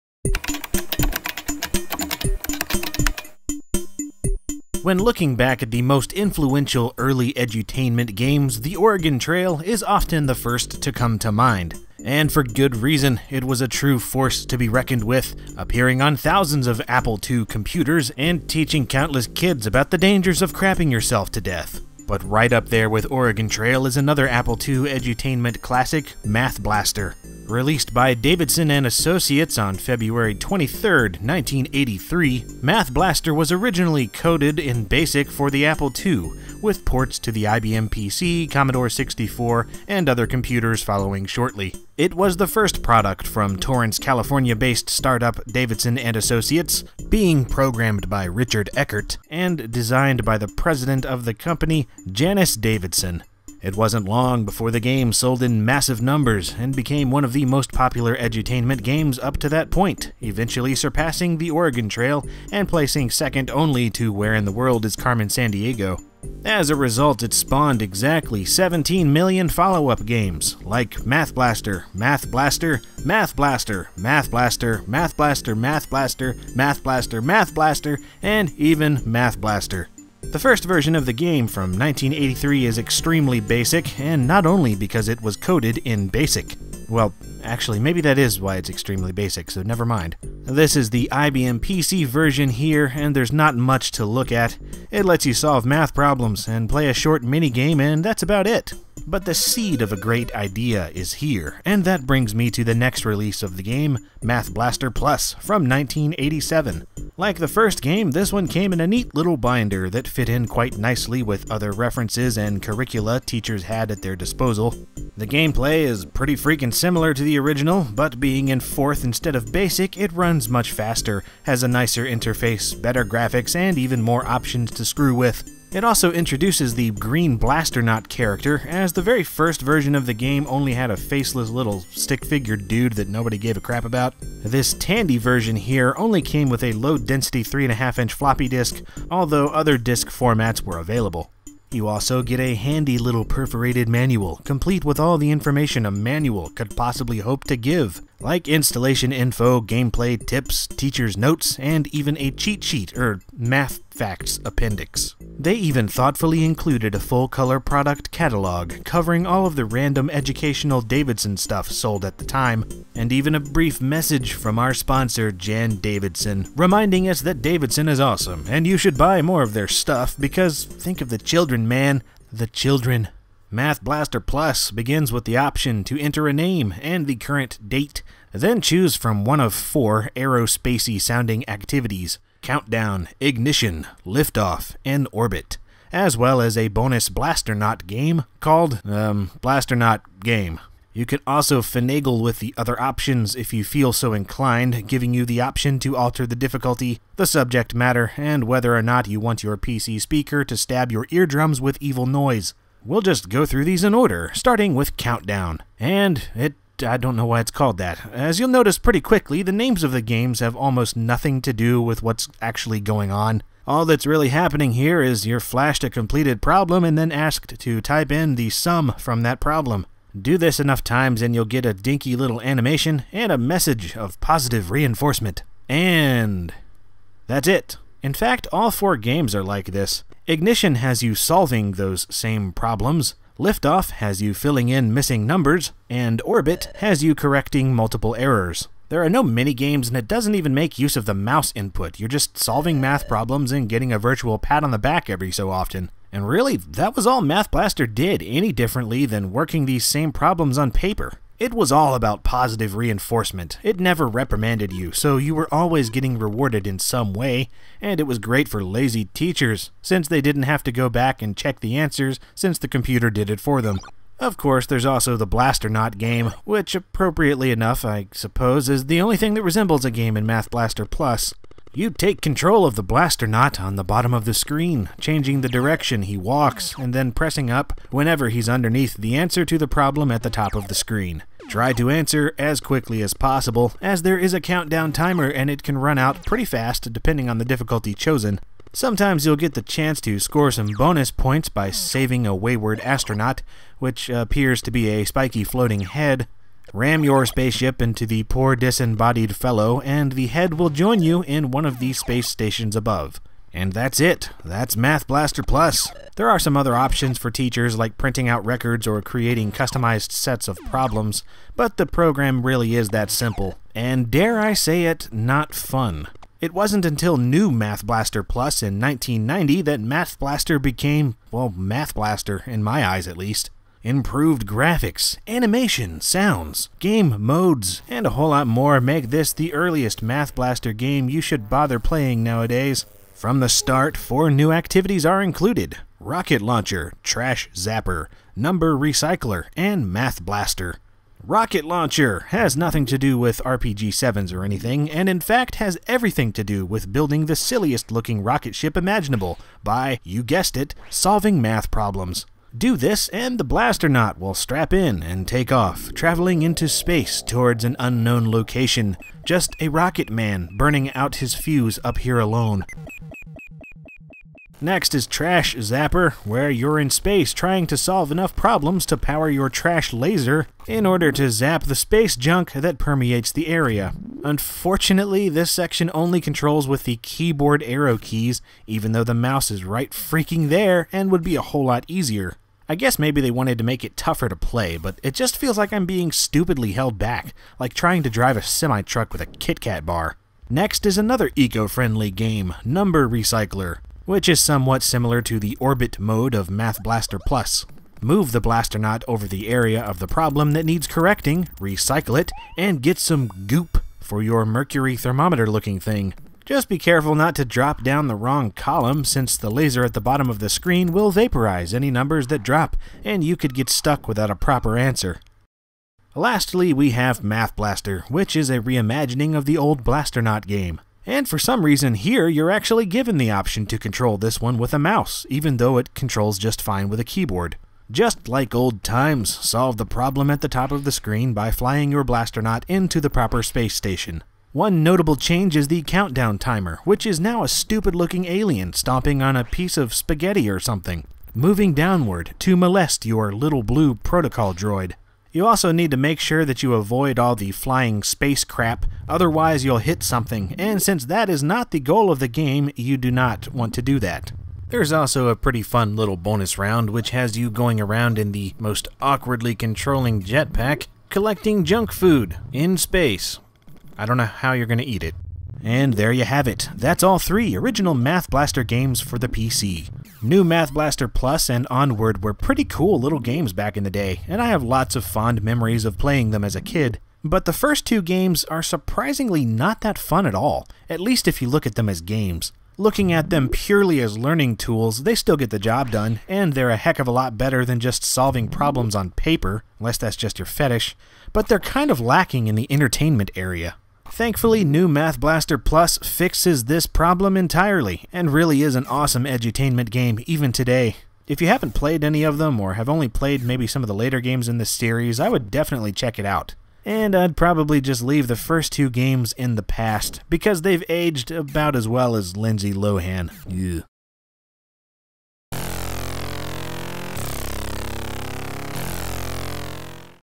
LGR - Math Blaster - PC Game Review
Aw yeah, time to blast some MATH! It's the Blasternaut to the rescue in this timeless edutainment classic for the Apple II, IBM PC, Commodore 64, and plenty more.